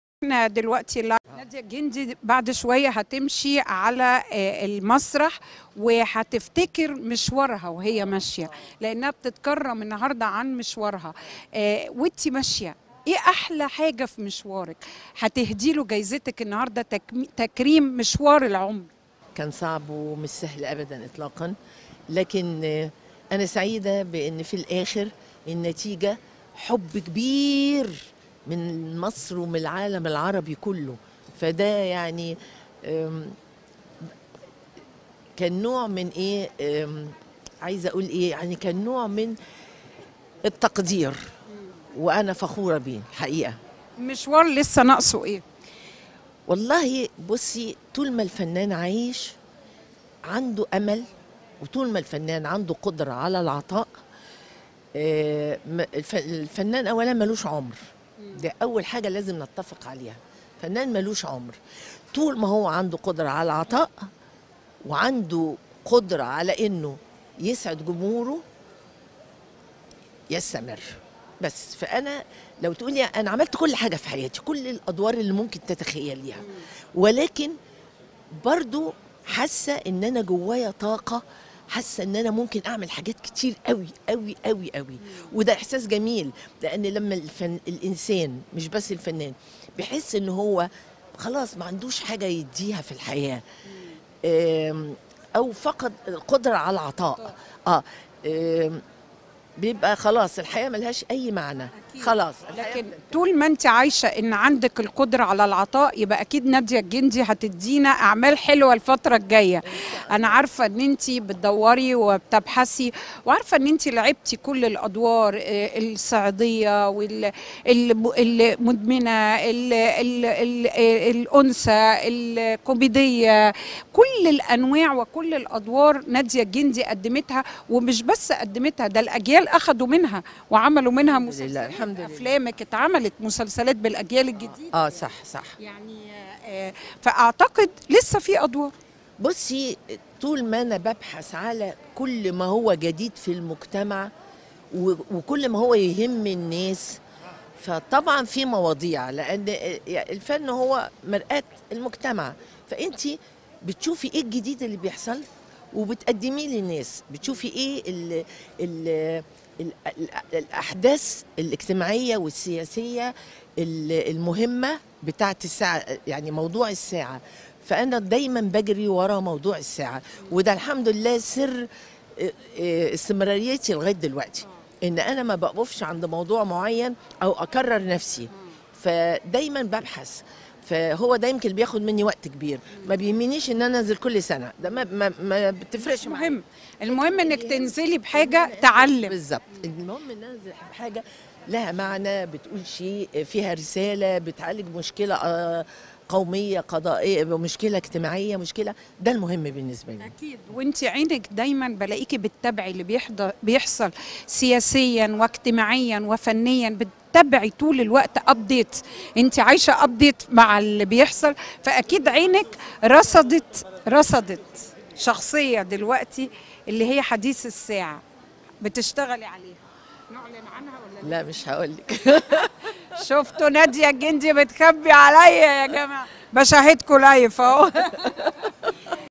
بوسي شلبي سألت نادية الجندي "مشوارك لسة ناقصه أية؟" شوف إجابتها كانت إية؟
ليالي_TeN - ردت نجمة الجماهير نادية الجندي، خلال لقائها مع الإعلامية بوسي شلبي، على هامش فعاليات توزيع جوائز مهرجان "الميروكس دور" في دورته العشرين في لبنان، على سؤالها "مشوارك لسة ناقصه أية؟"